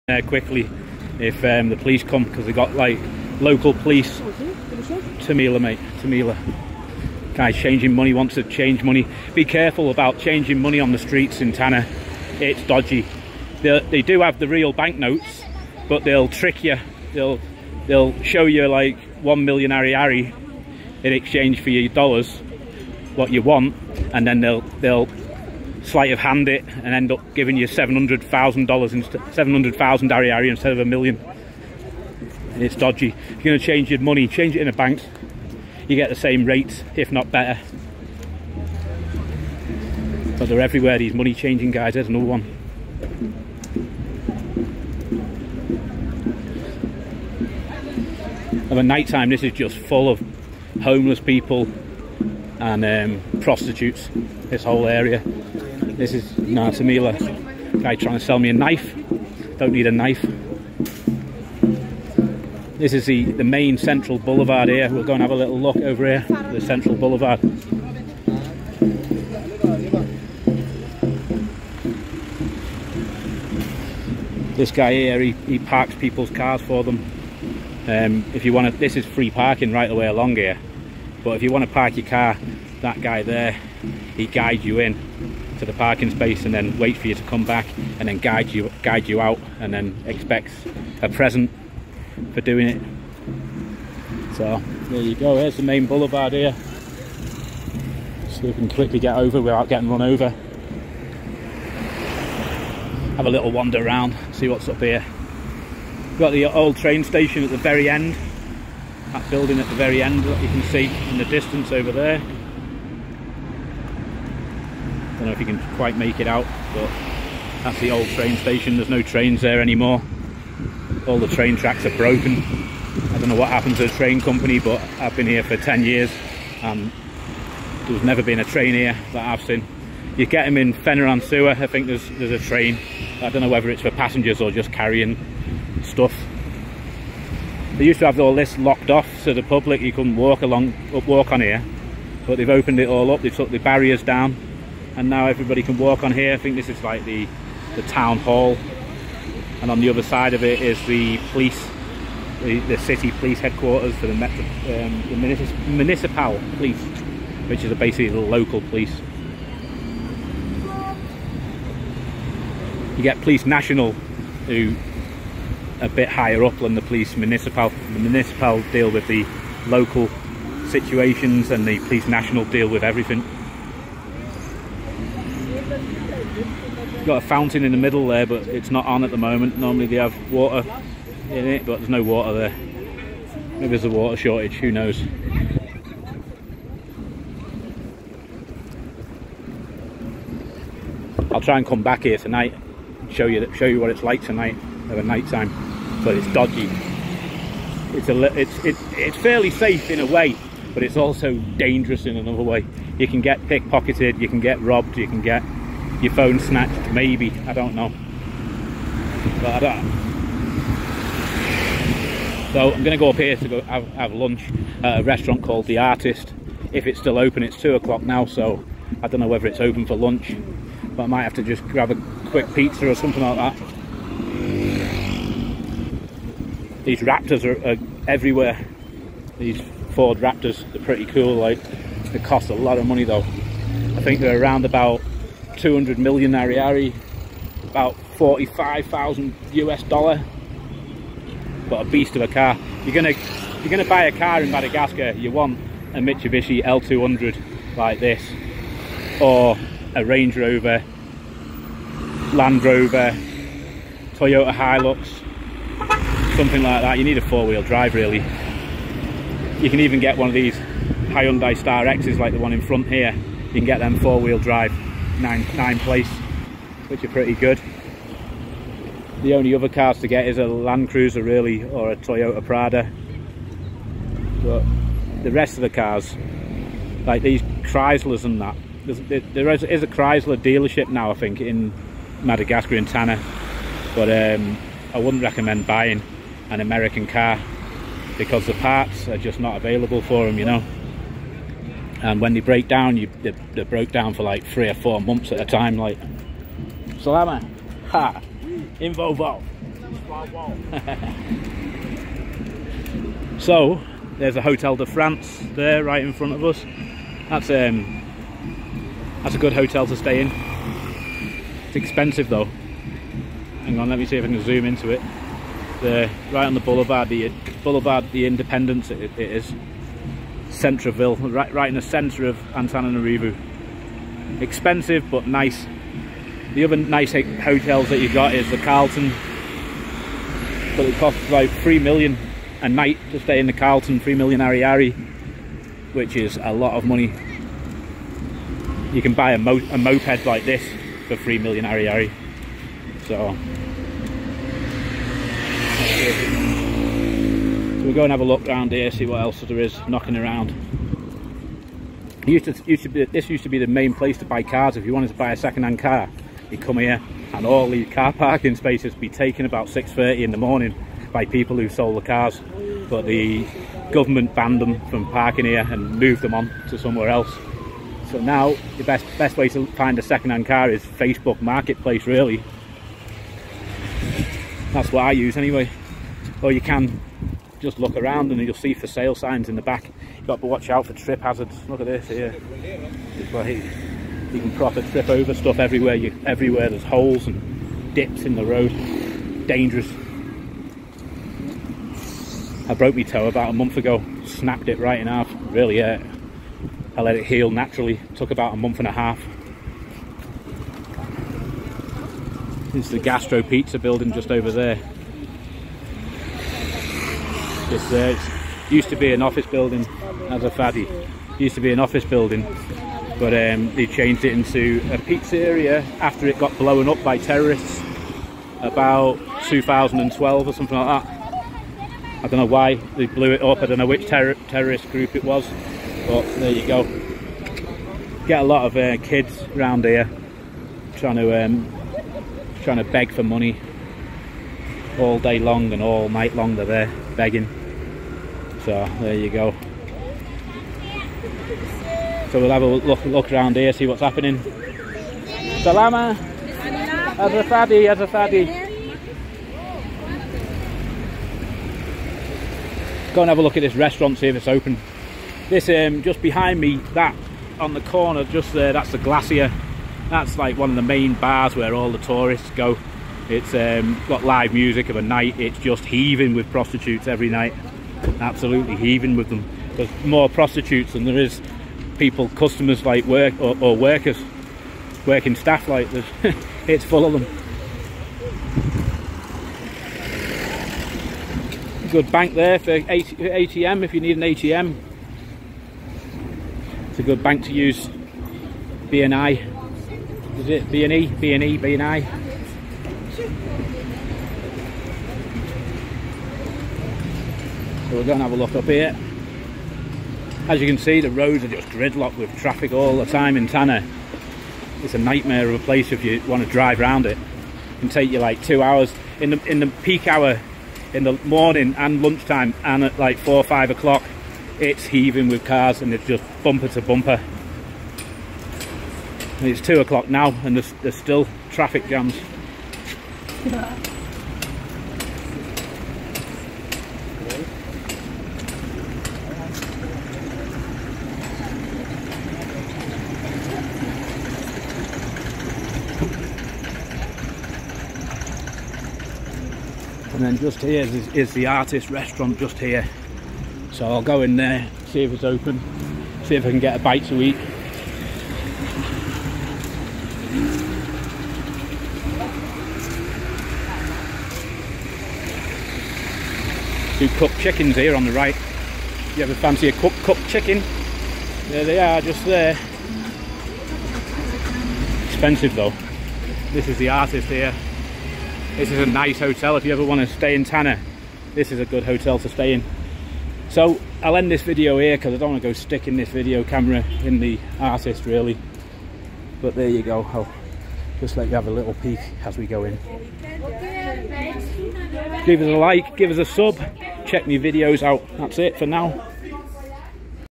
Antananarivo Madagascar
walking around Antananarivo Madagascar this afternoon